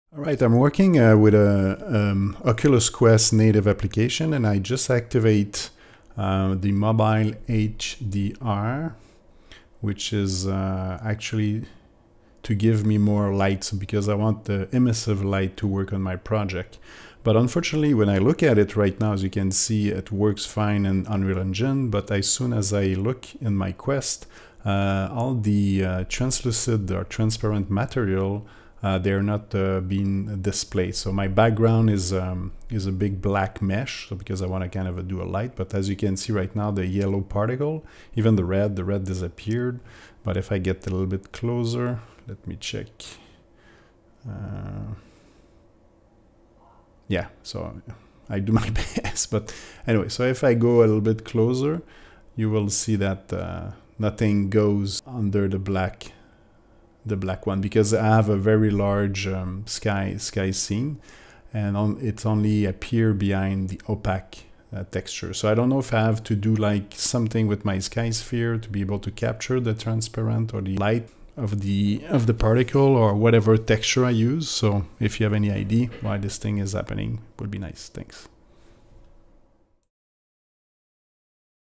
Unreal Engine 4/27 with Mobile HDR for Oculus Quest 2 using Transparent Material do not work
I create a Transparent Material in Unreal Engine and I activate Mobile HDR to get emissive light. I create a large Black SkySphere but unfortunately, it does not work well on Oculus. Any suggestions ?

Note: I know Mobile HDR sucks on Oculus Quest 2 but I still want to get emissive light.